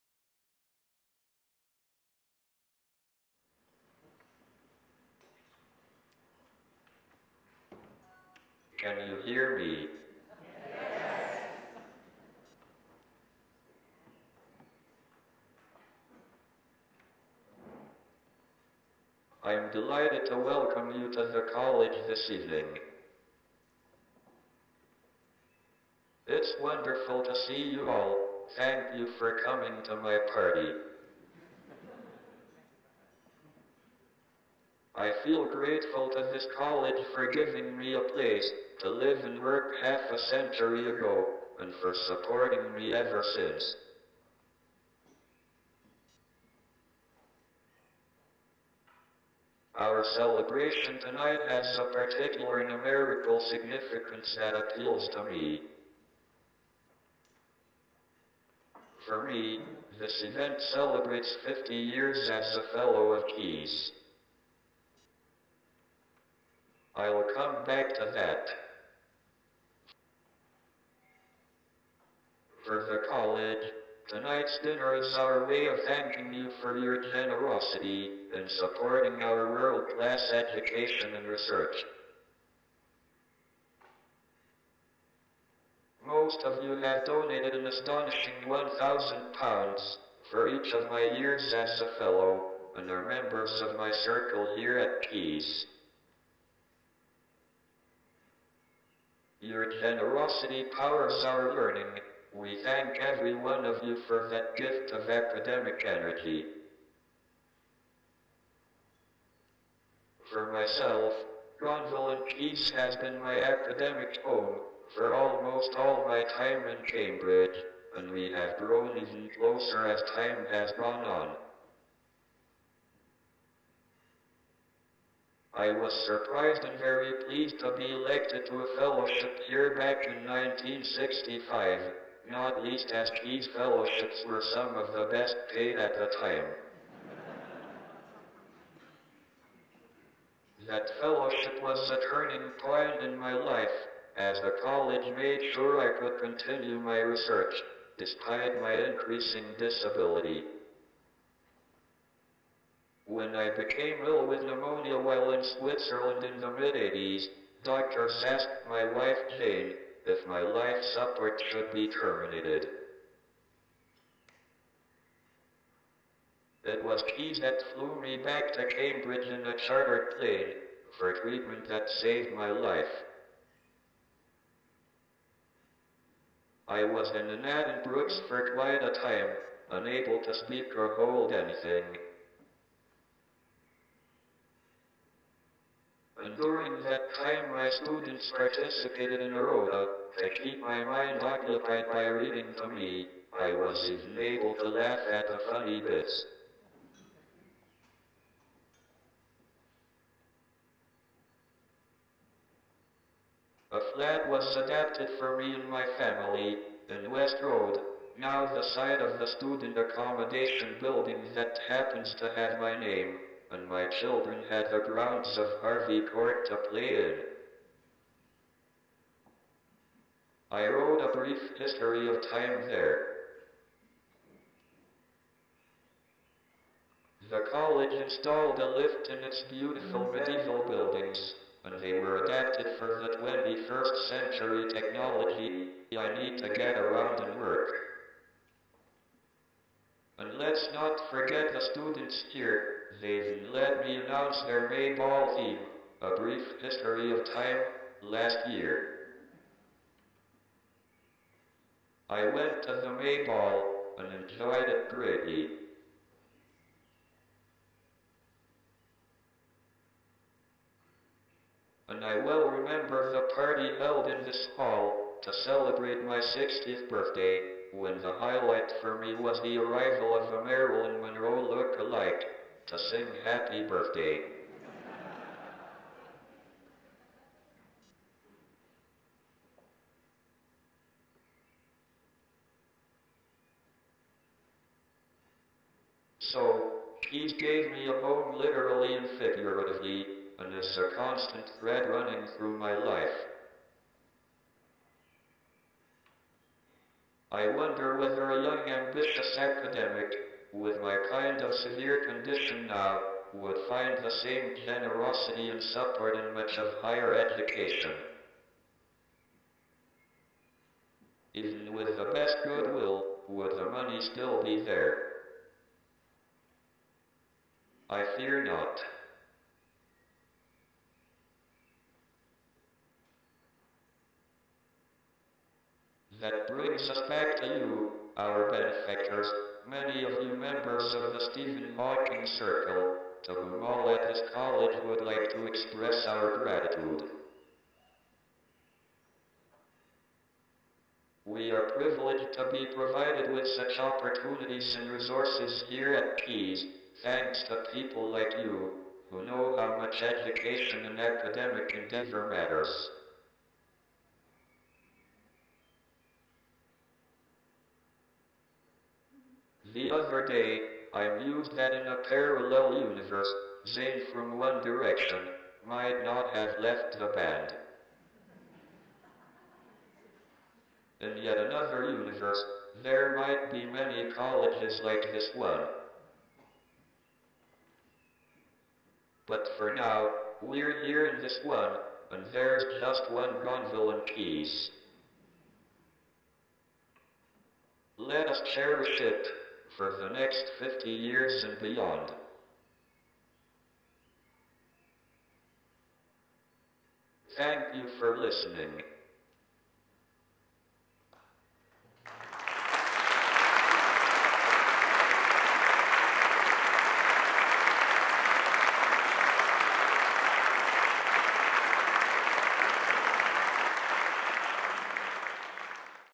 Prof Stephen Hawking pays tribute to his college Gonville & Caius
Speech by Prof Stephen Hawking thanking his college Gonville & Caius for support over his 50 years as a Fellow, and questioning whether such support is available for disabled students in higher education today.